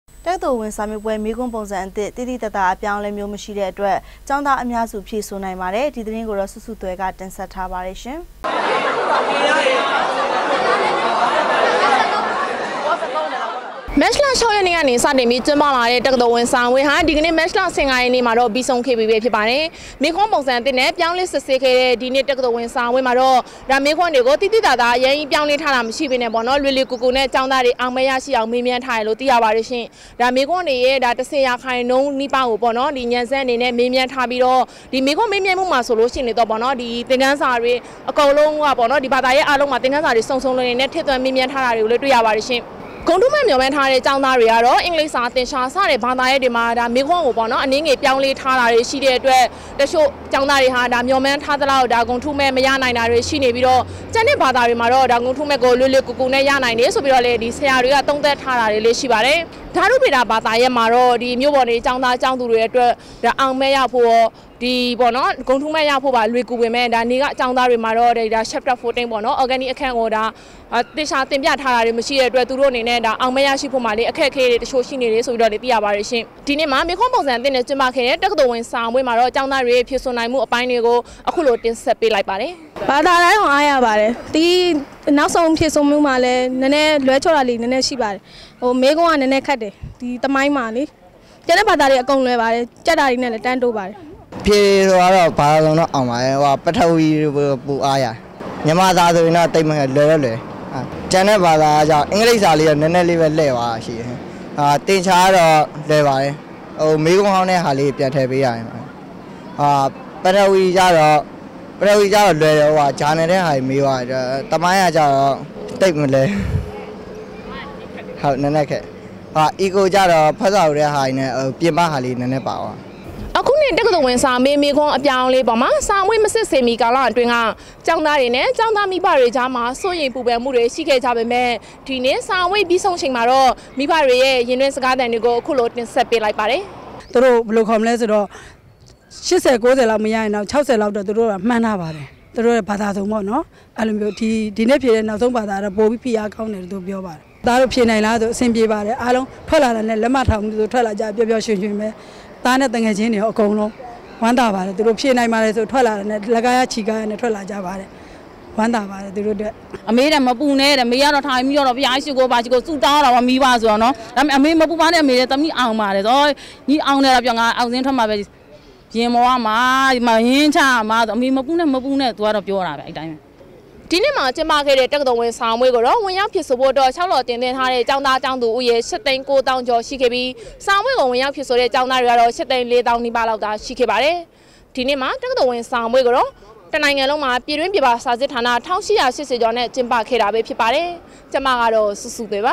DVB - တကၠသိုလ္ဝင္စာေမးပြဲ ေမးခြန္းပံုစံအသစ္ ေက်ာင္းသားအမ်ားစု ေျဖဆိုနိုင္
တကၠသိုလ္ဝင္စာေမးပြဲ ေမးခြန္းပံုစံအသစ္ သိသိသာသာအေျပာင္းအလဲမ်ိဳး မရွိတဲ႔အတြက္ ေက်ာင္းသားအမ်ားစု ေျဖဆုိႏုိင္ခဲ့ပါတယ္။ ဒီသတင္းကိုဆုဆုသြယ္ကတင္ဆက္ထားပါတယ္။
DVB TV -